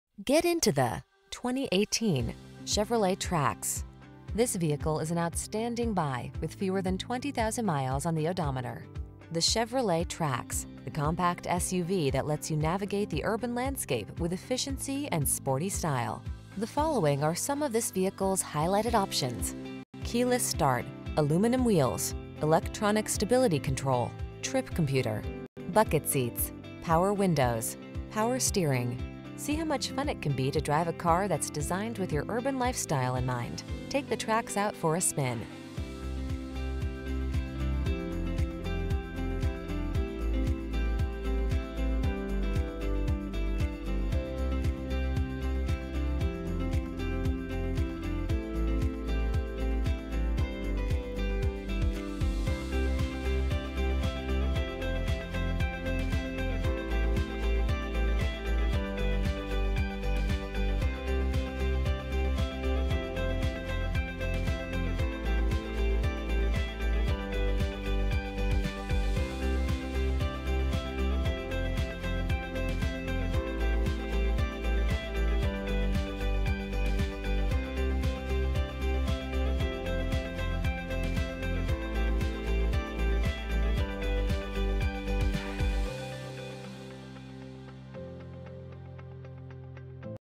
2018 Chevrolet Trax Lewisville, Dallas, Carrollton, Richardson, TX PX238411A
Silver Ice Metallic Used 2018 Chevrolet Trax available in Lewisville, Texas at Lone Star Toyota. Servicing the Dallas, Carrollton, and Richardson, TX area.

1547 S. Stemmons Fwy

Silver Ice Metallic 2018 Chevrolet Trax LT 4D Sport Utility 6-Speed Automatic ECOTEC 1.4L I4 SMPI DOHC Turbocharged VVTbrbrPriced below KBB Fair Purchase Price! Odometer is 28978 miles below market average! 25/33 City/Highway MPG Recent Arrival!brbrbrCARFAX One-Owner. Clean CARFAX.brbrbrKBB Fair Market Range High: $22027 KBB Fair Market Range Low: $19159brbrbrAwards:br  * 2018 KBB.com 10 Most Awarded Brands